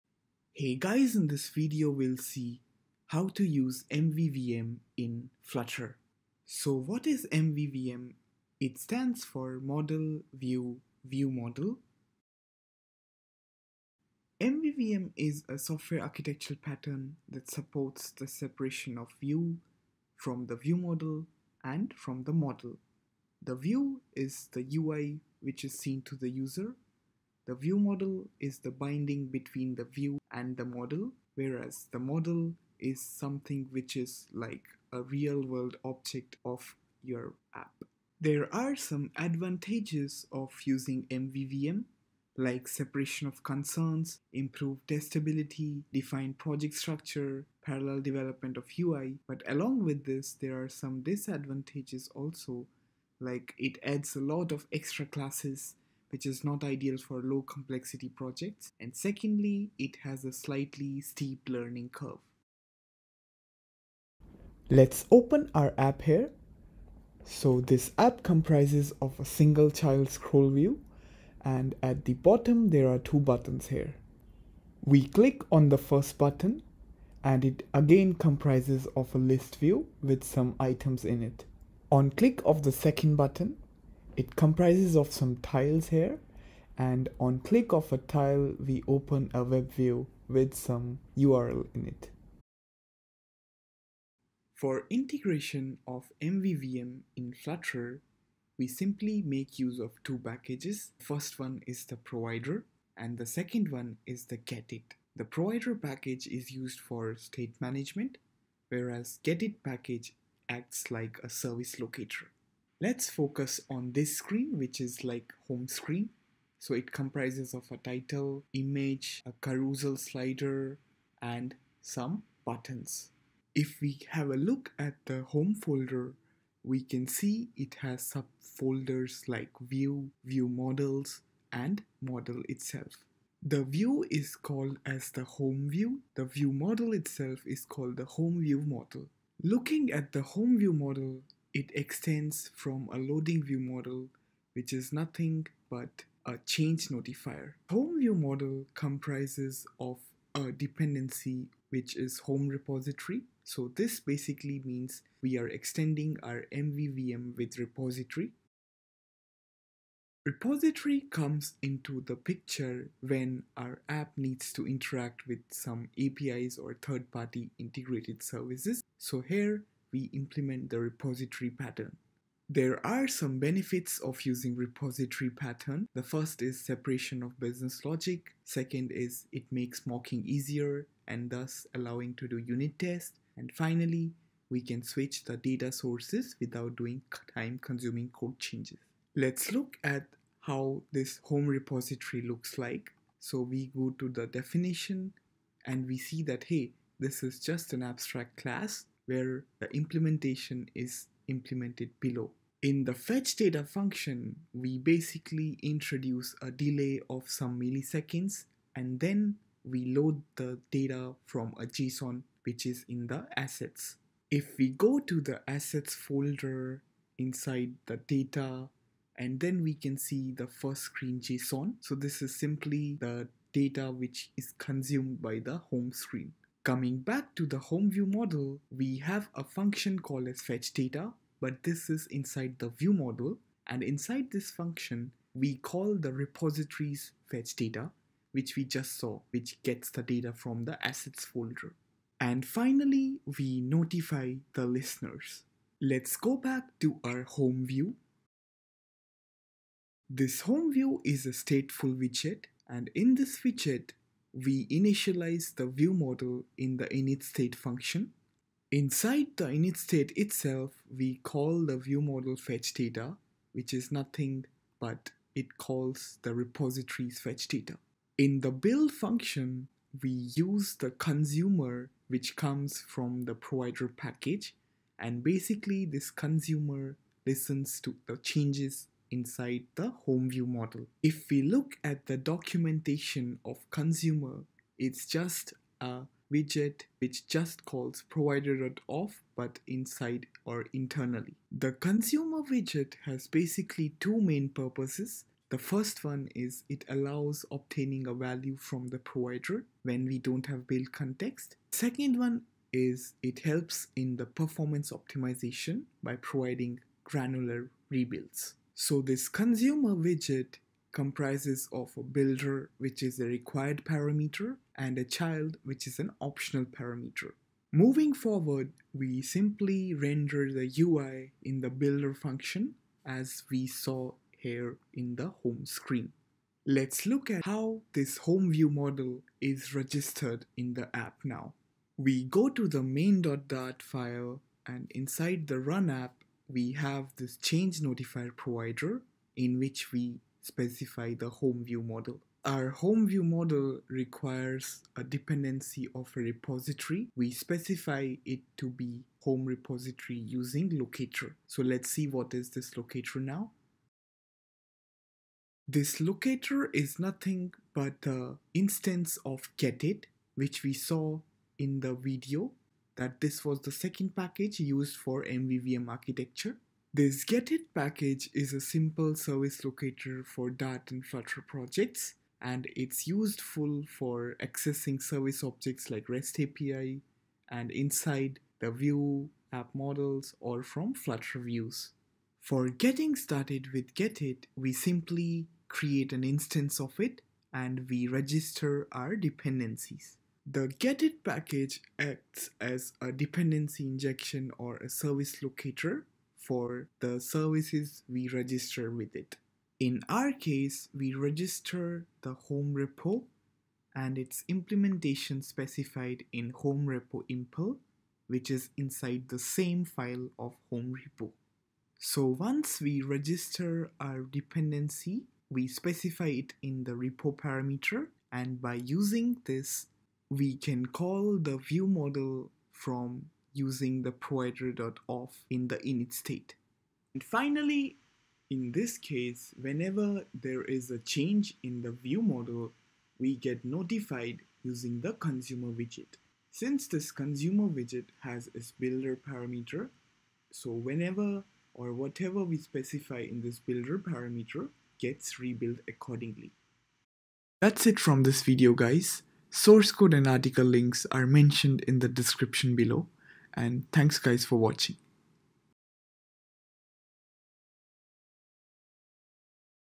MVVM in Flutter | How to use MVVM in Flutter (2022) | Architecture Flutter app
Model-View-ViewModel (MVVM) is a software architectural pattern that supports the separation of the UI (which is View) from the development of the business logic or the backend logic (Model). The view model inside MVVM is the bridge responsible for the conversion of data in a way that behaves in accordance with the changes happening on the UI

* MVVM in Flutter | How to use MVVM in Flutter (2022) *

0:12 About MVVM
1:33 Setup MVVM
2:39 MVVM + Repository Pattern
4:08 Integrate with UI
5:54 Using GetIt for DI (Dependency Injection)